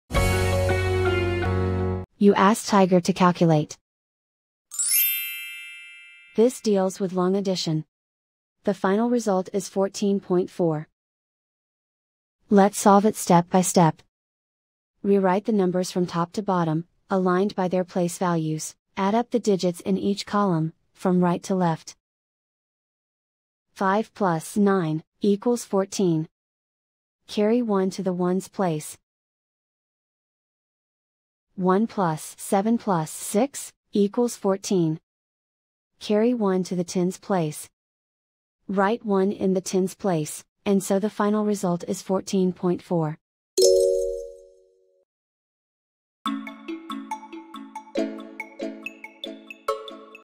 Long Addition Problem 7.5+6.9: Step-by-Step Video Solution | Tiger Algebra
See how to easily solve the addition problem 7.5+6.9 with our clear, step-by-step video guide on Tiger Algebra.